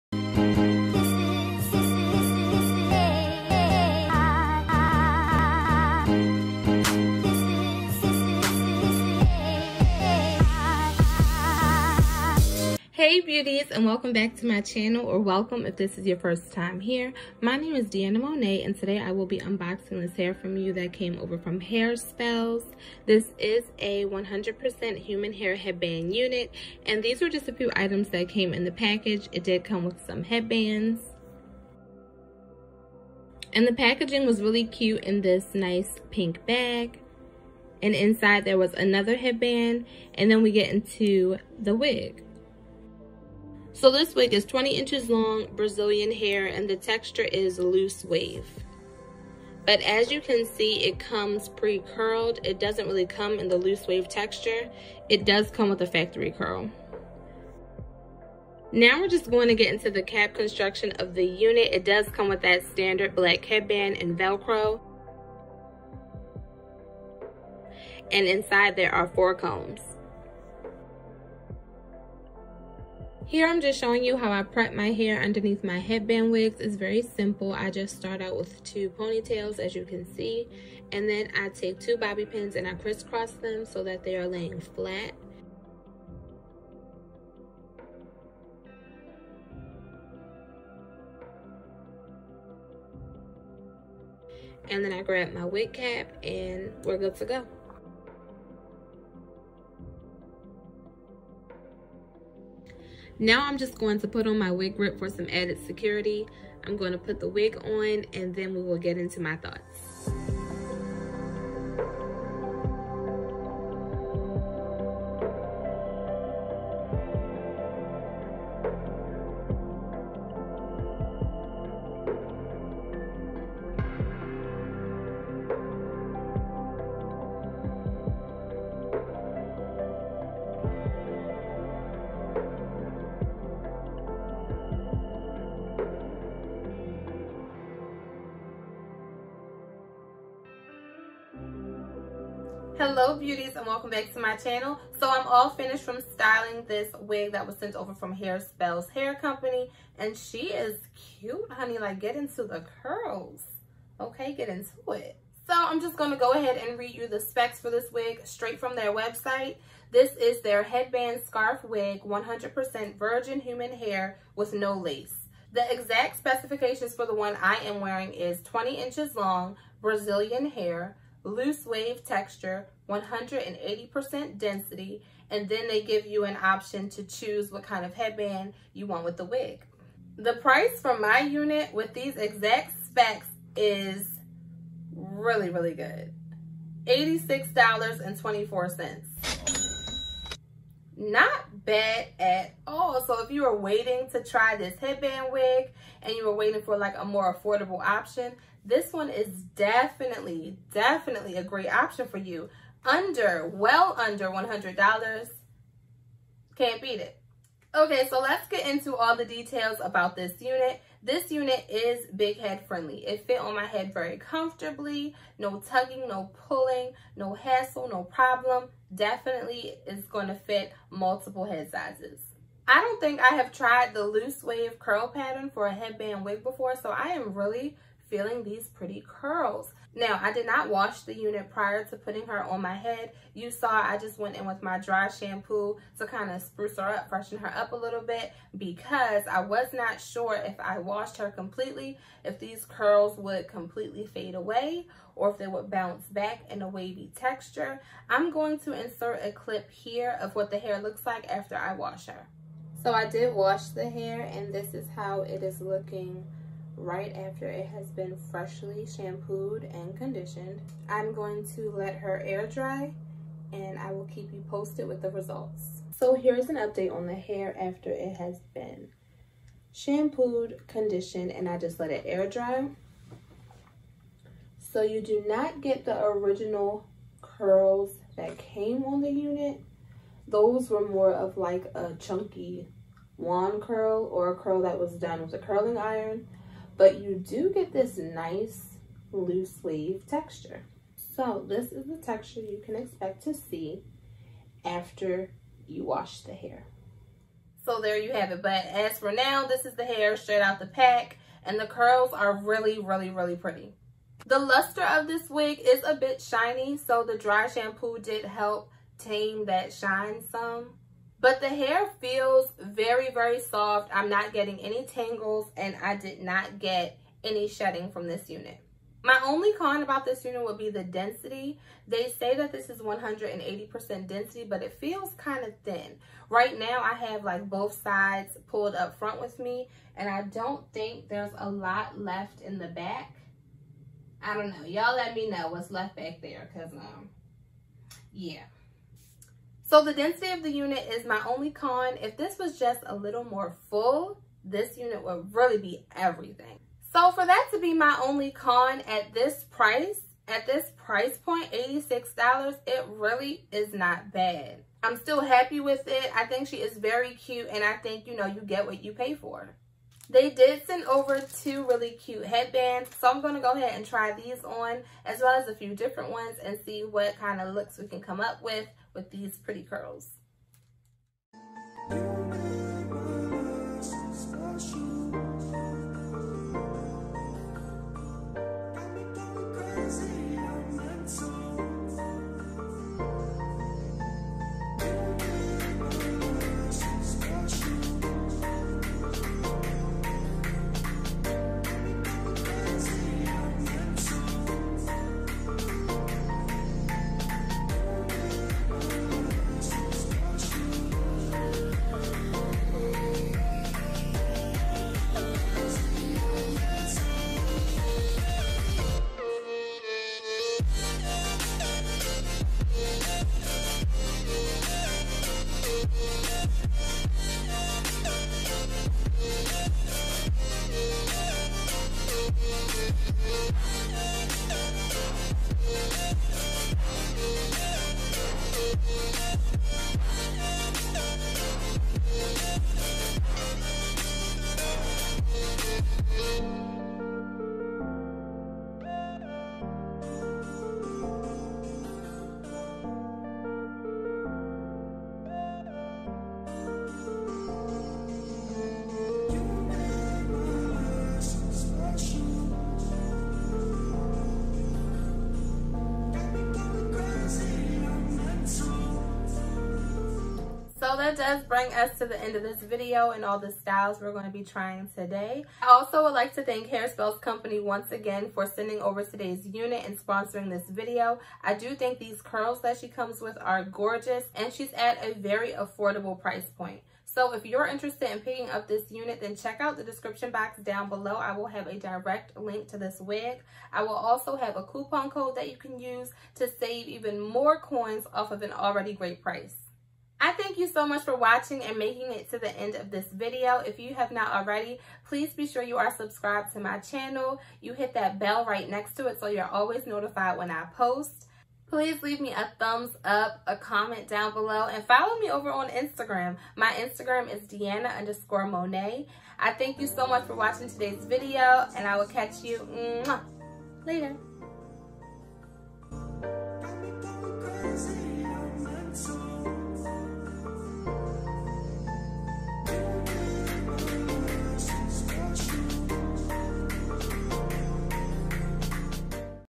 Hairspells Headband Wig Review Under $90!
Wig Details: 180% density Brazilian loose wave headband scarf wig 20 inches